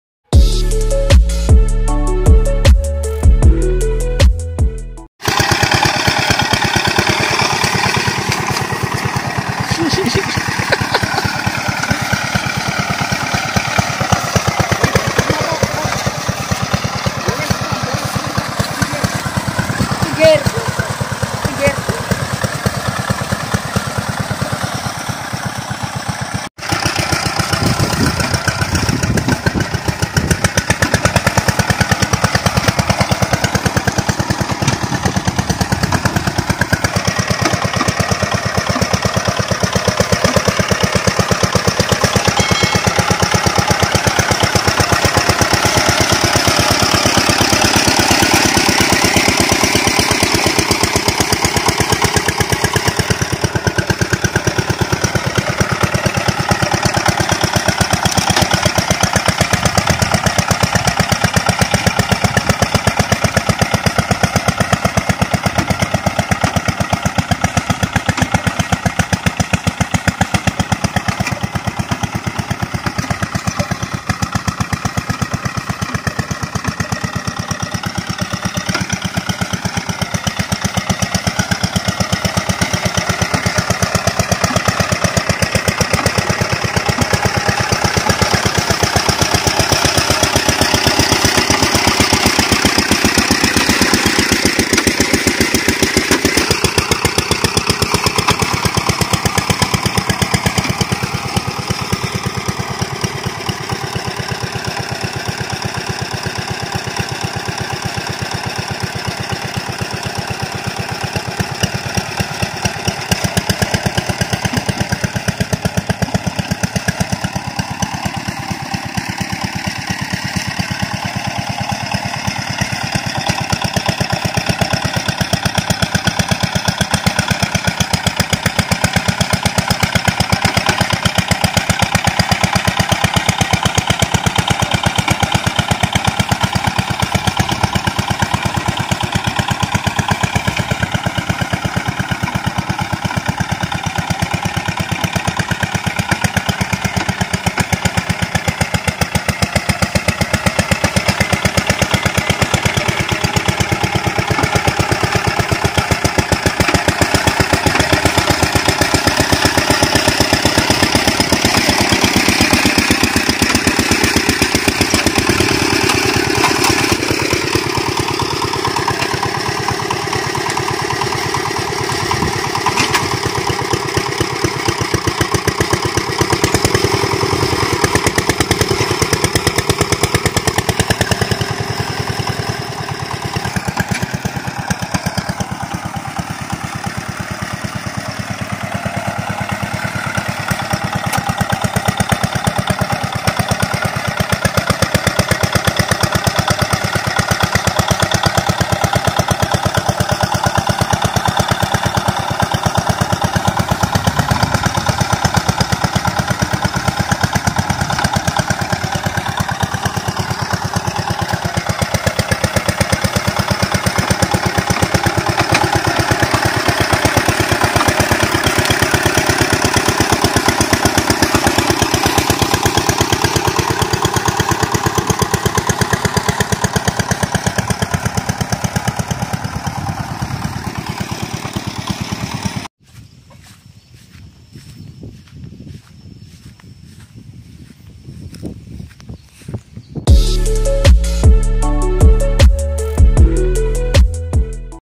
Membajak Sawah
Membajak Sawah adalah menggemburkan tanah sebelum melakukan penanaman tanaman